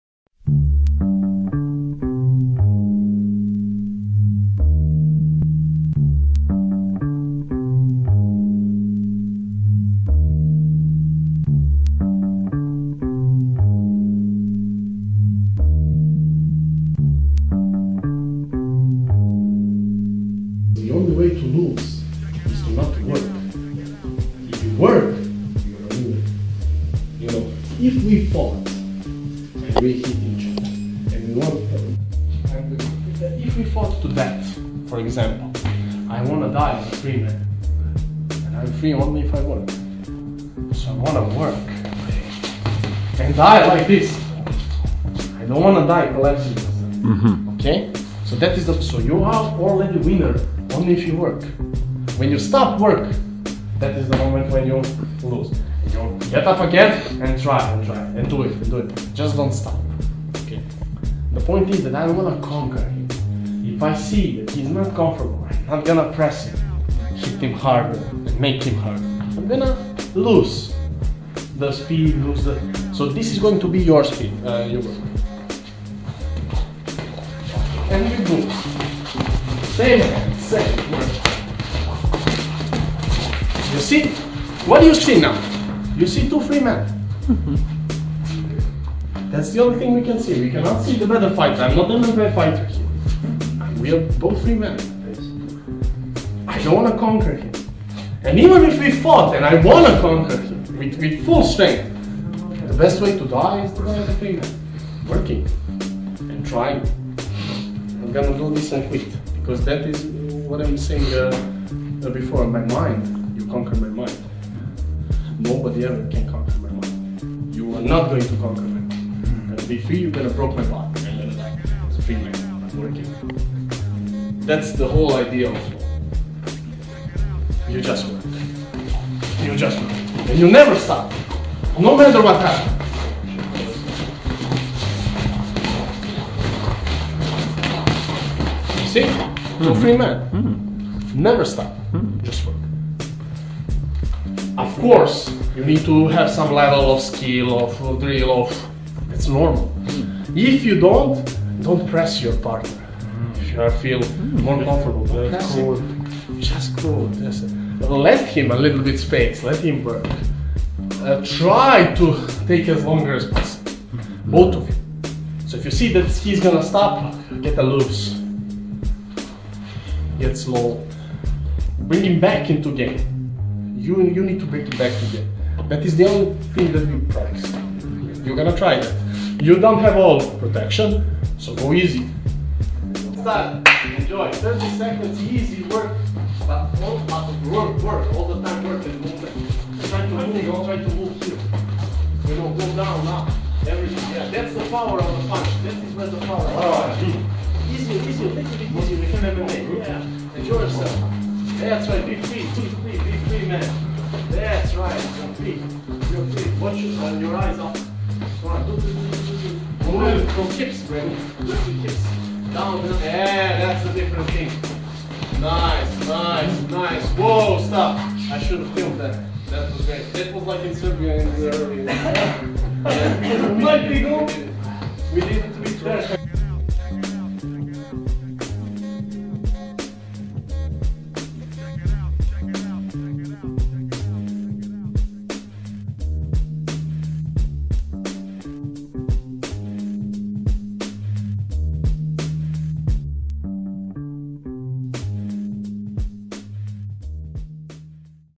HOMOLUDENS - Soft boxe flow *Fight, live or die but like a freeman*
Excerpt from Serbia seminar november 2017.
A soft exercise of boxe flow "Fight, live or die but like a freeman".
From Milos Markovic Pro MMA fighter.